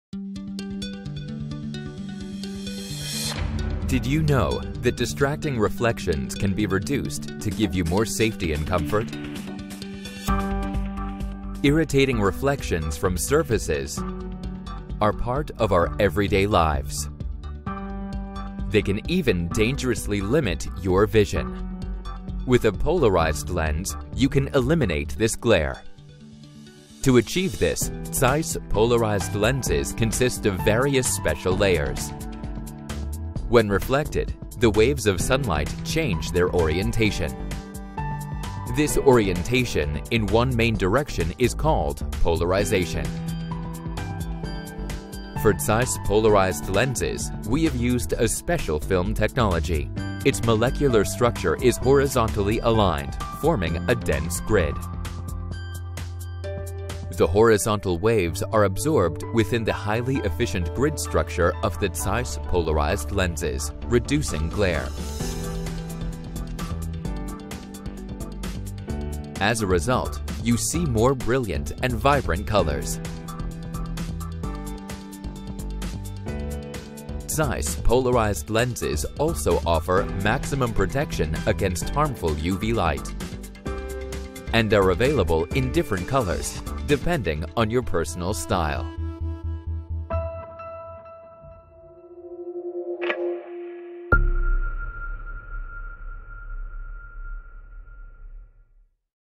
How Do ZEISS Polarized Lenses Work?
Polarized lenses reduce reflected light that irritates the eye. See how ZEISS polarized lenses work, and how they can help you see better through your daily activities - whether you're driving, skiing, relaxing at the beach, or playing sports. ZEISS polarized lenses come in a variety of colors to suit your style and offer 100% UVA and UVB protection.